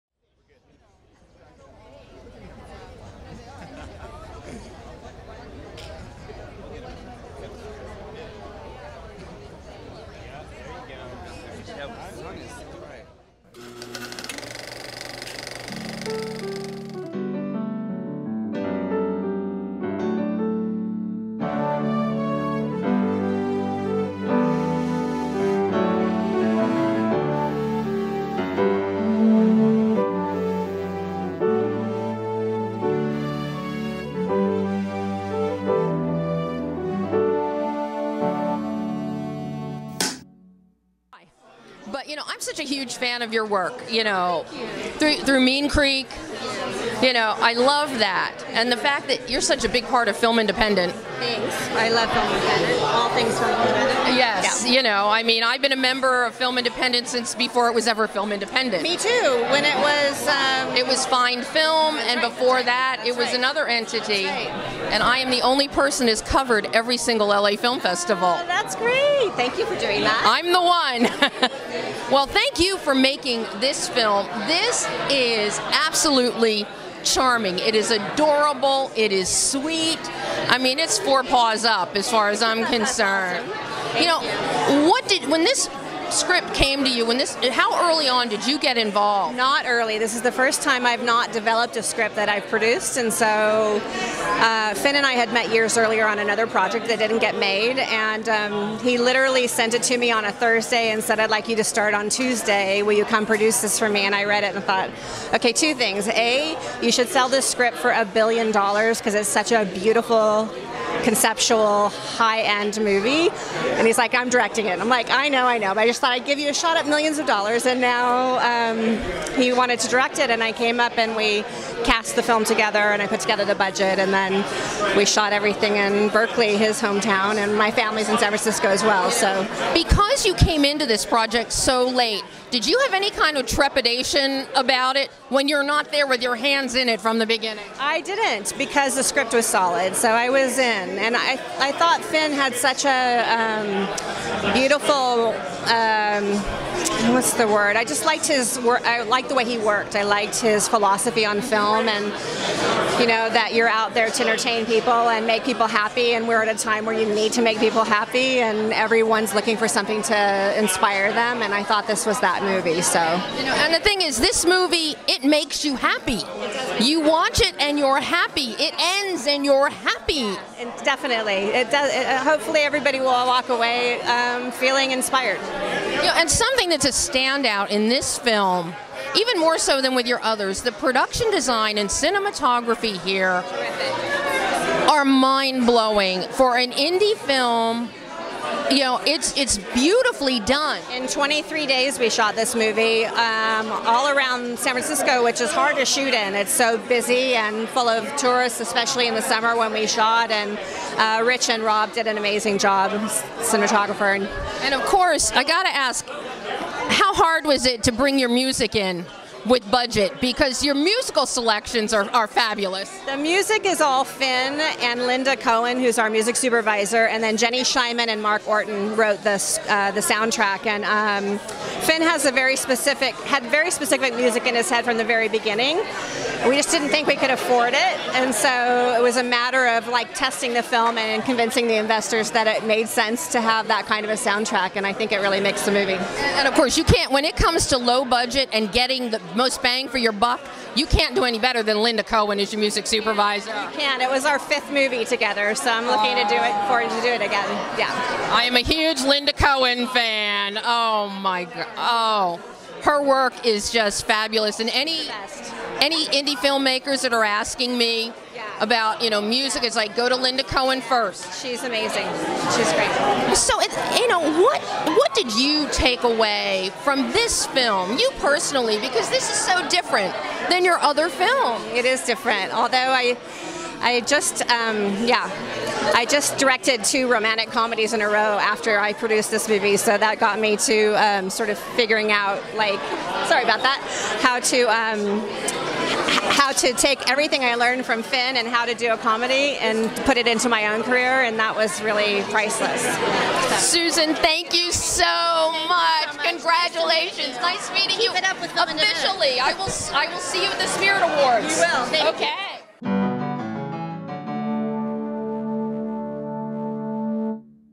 UNLEASHED Premiere - Carpet Chat with SUSAN JOHNSON
Carpet Chat with SUSAN JOHNSON, producer of UNLEASHED.

The magic was "unleashed" at the Laemmle Santa Monica with the theatrical premiere of the adorably sweet rom-com of a different species - UNLEASHED.

Cosmic forces aligned as filmmakers, producers, stars and celebrity guests hit the red carpet to chat with film critic debbie elias before the premiere full house.